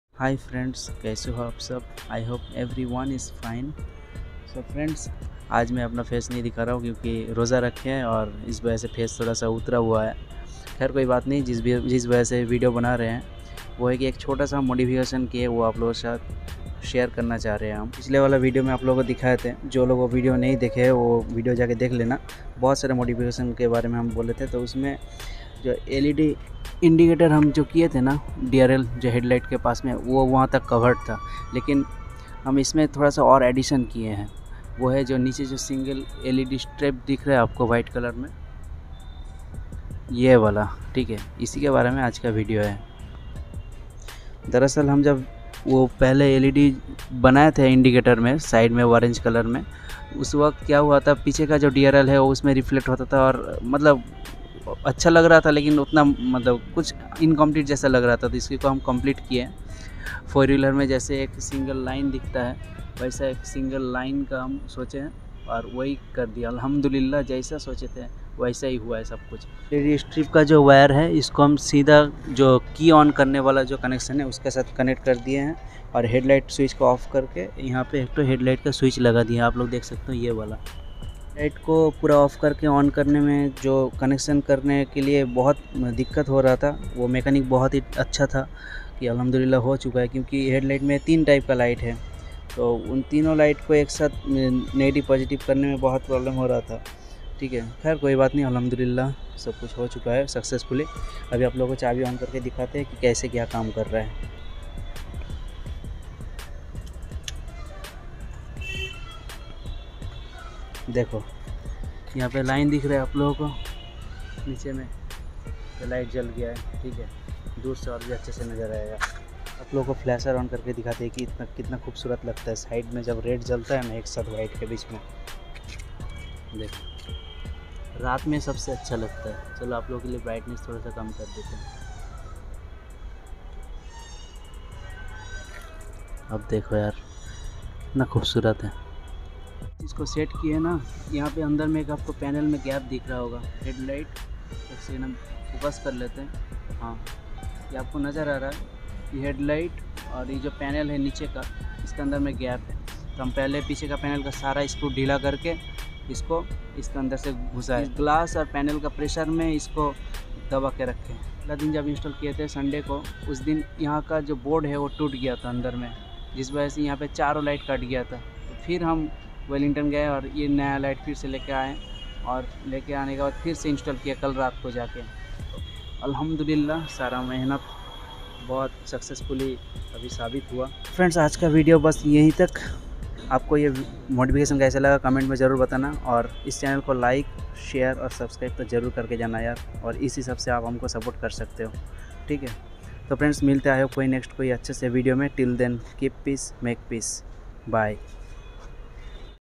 R15 v3 Headlight DRL with Indicator Modification | First Time in India
This video is about modifications of headlight drl and indicators in headlight of my R15v3.